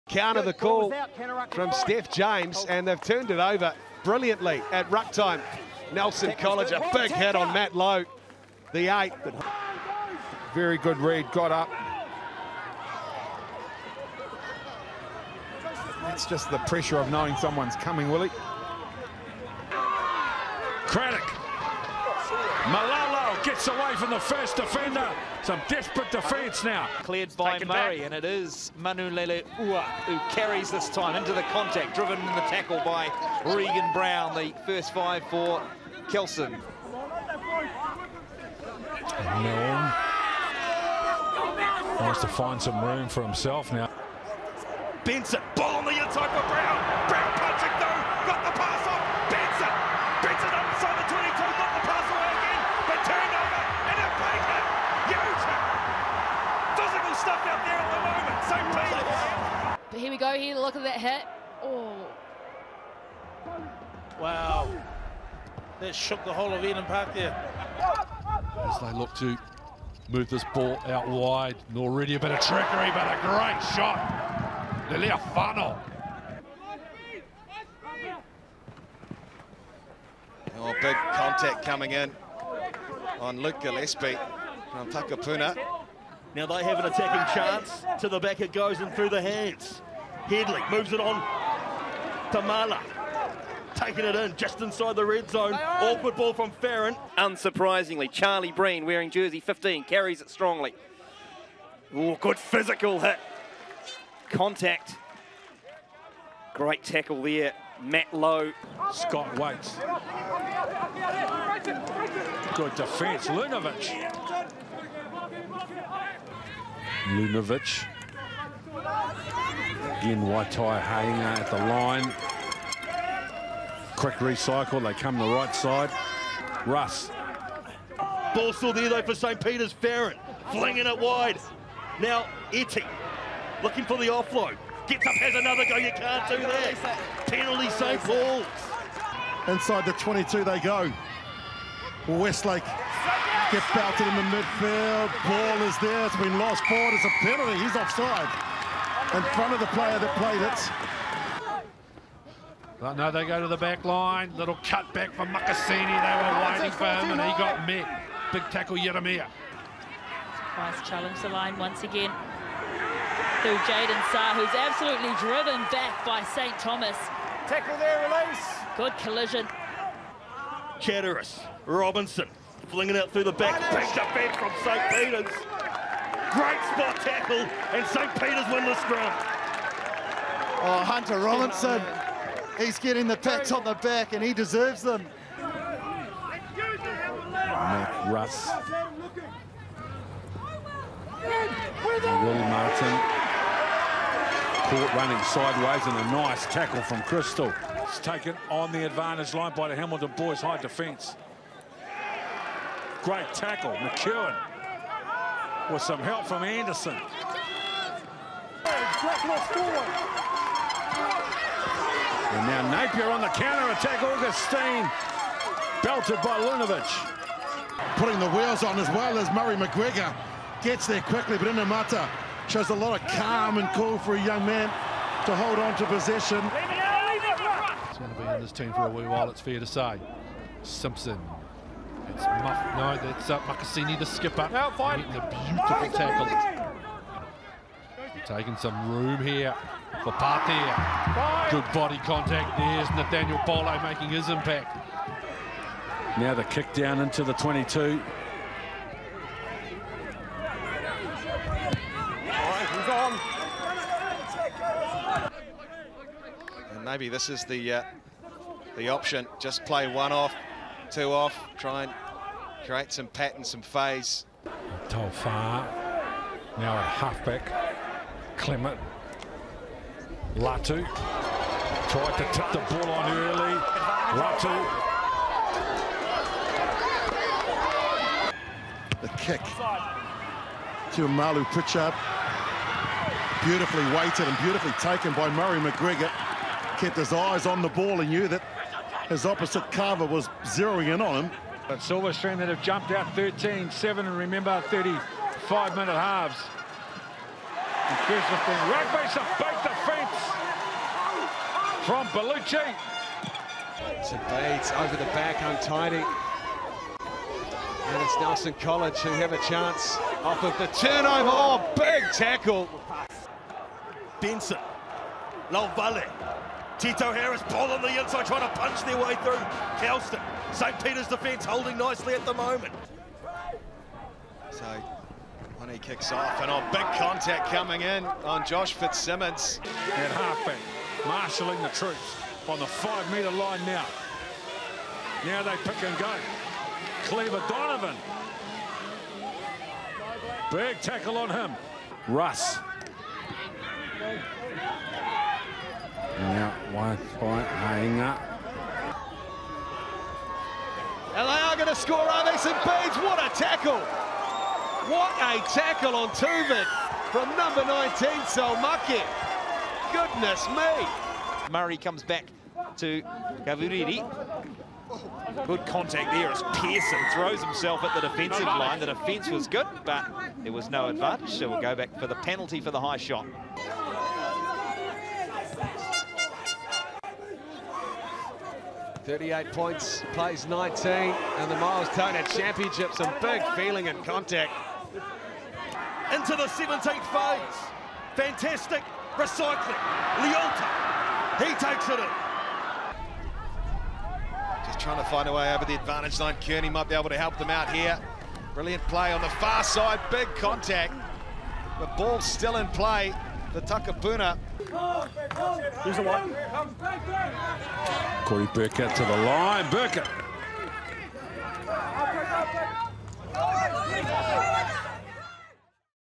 The defence in New Zealand schoolboy rugby is outstanding | 1st XV tackling highlights
Rugby highlights showcasing the high quality of defence in schools rugby in New Zealand. This video showcases some of the top tackles from schools rugby in New Zealand rugby.